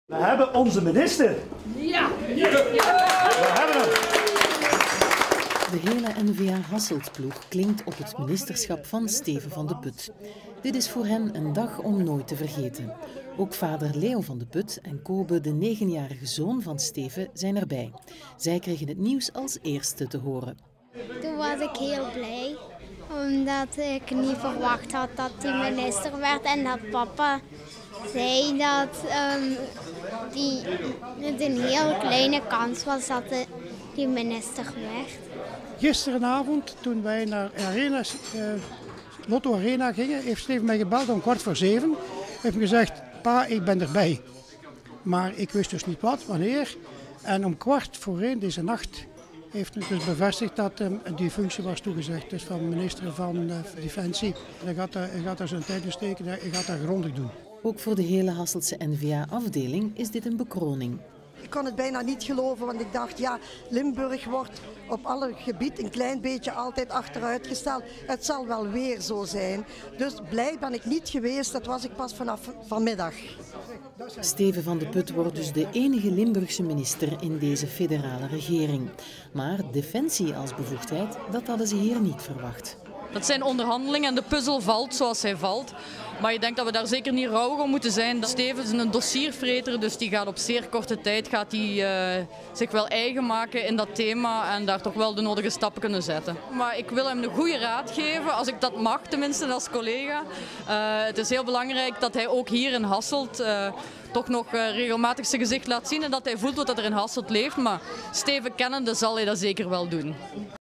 Achterban van Steven Vandeput feest
De benoeming tot minister is echt een droom die uitkomt, dat zegt de vader van Steven, Leo Vandeput. Hij vierde deze middag samen met heel wat andere Hasseltse N-VA-leden in hun stamcafé. En ook de negenjarige zoon van Steven Vandeput was erbij.